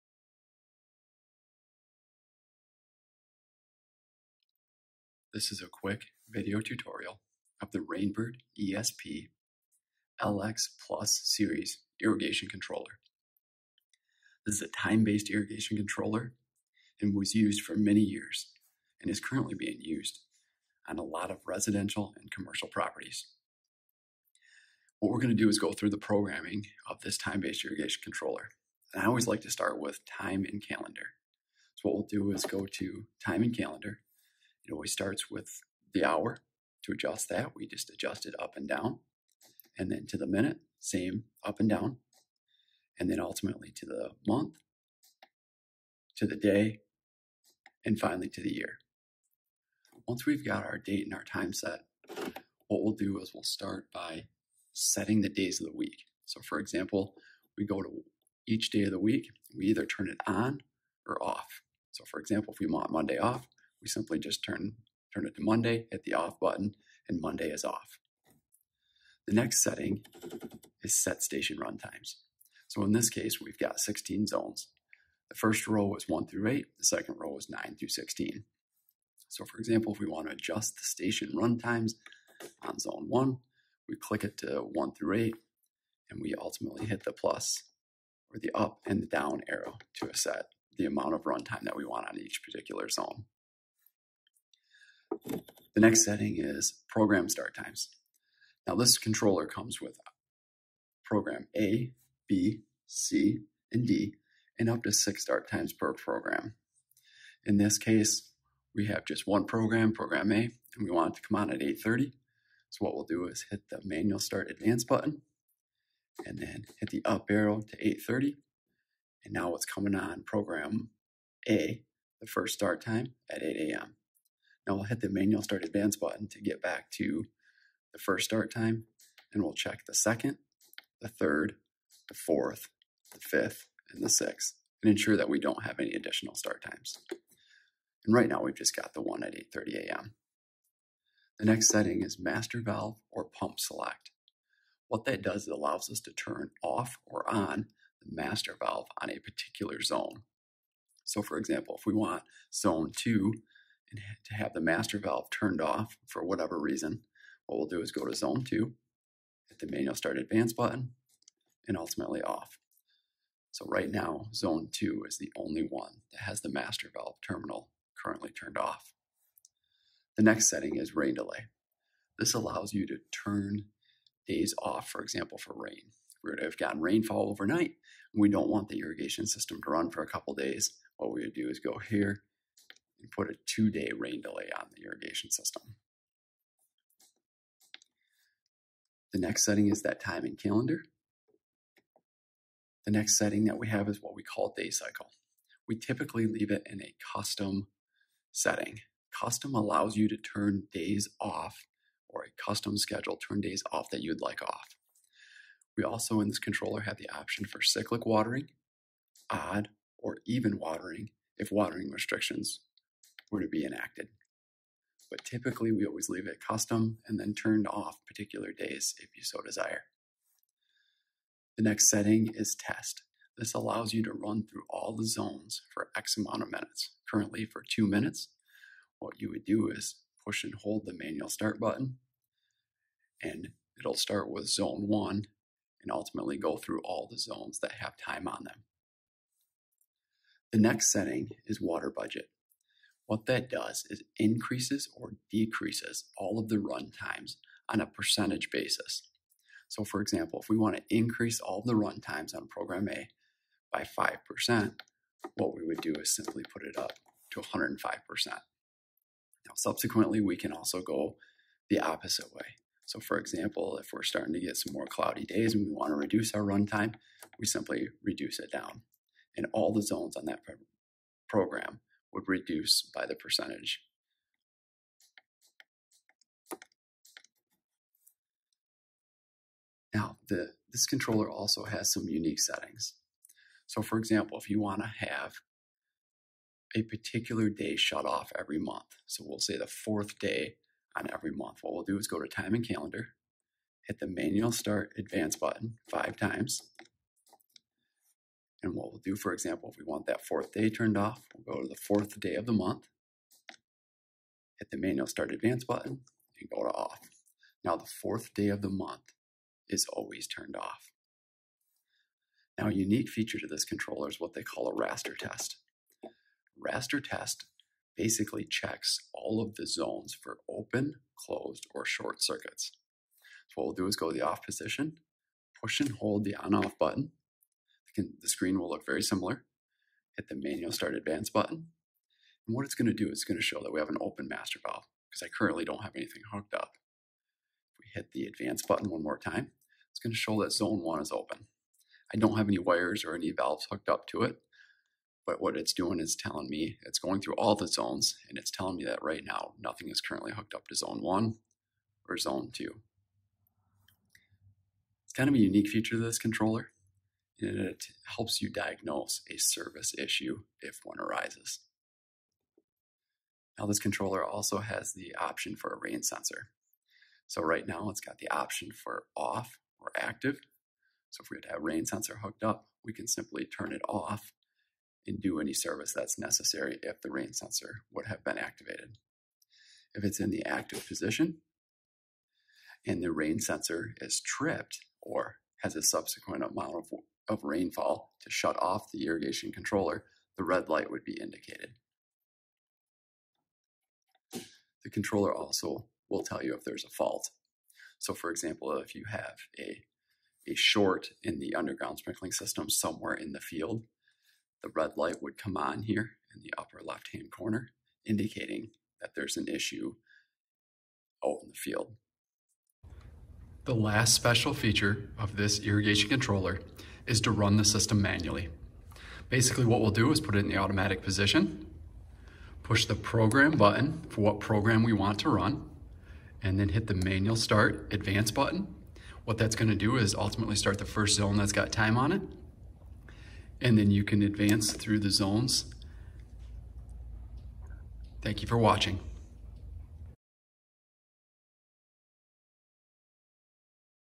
Rain Bird ESP LX Plus Irrigation Controller Programming
This is a quick video tutorial on programming the Rain Bird ESP-LX Plus irrigation controllers. The controllers was manufactured in 8,16,20, and 24 station models. The controller is used in residential and commercial applications.